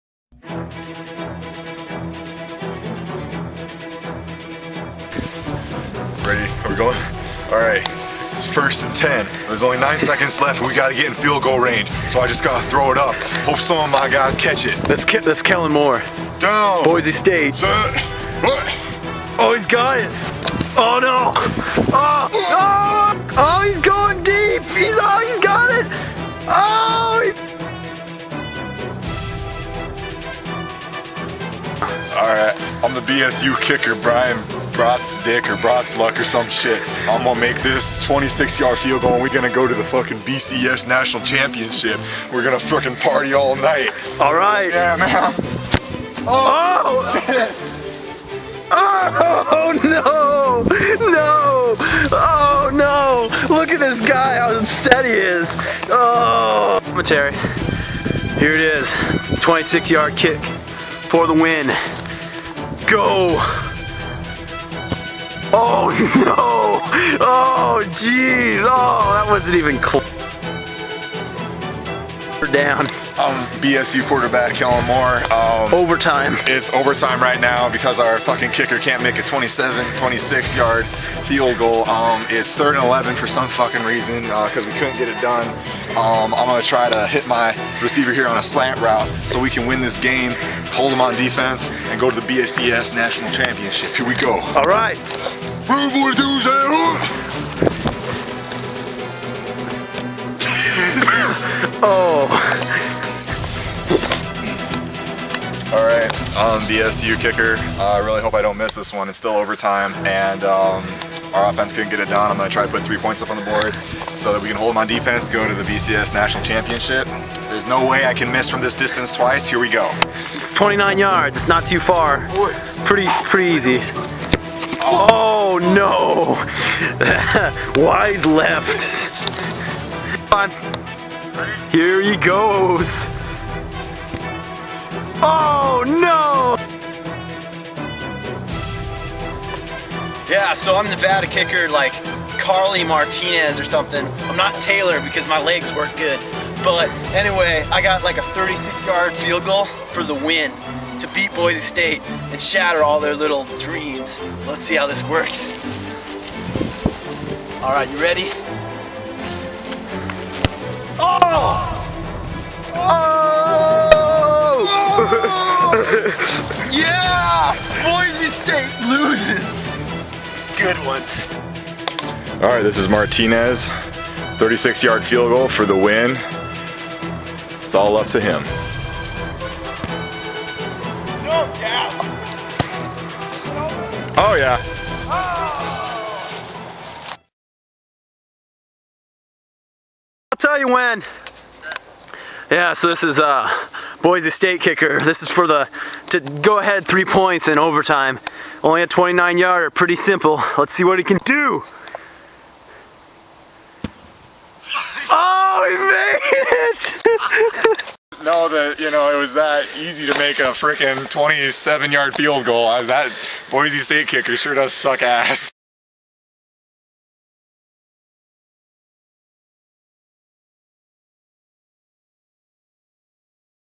Boise State vs. Nevada Highlights 2010
Nevada upsets Boise State in overtime with a field goal.